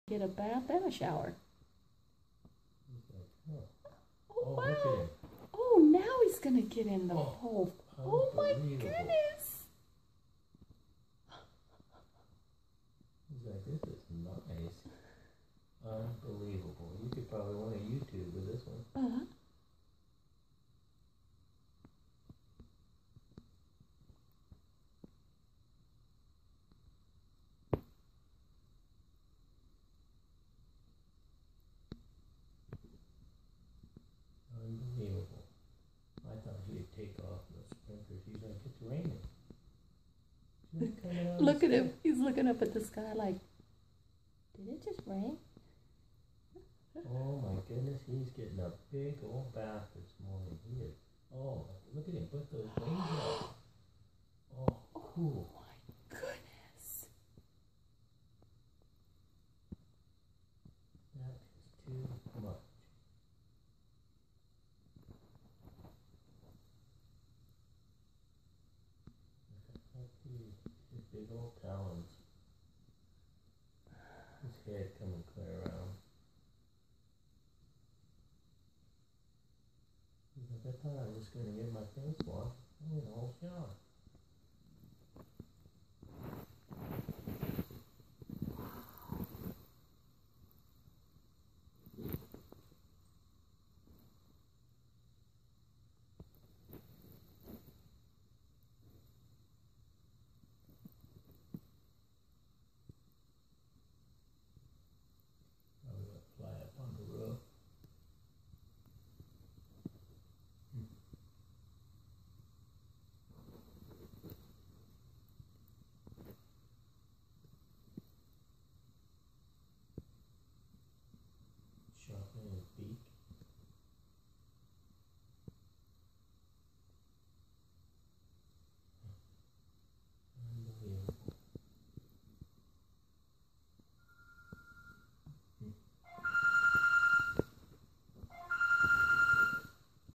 Owl in a Bird Bath
Owl in bird bath gets a shower too!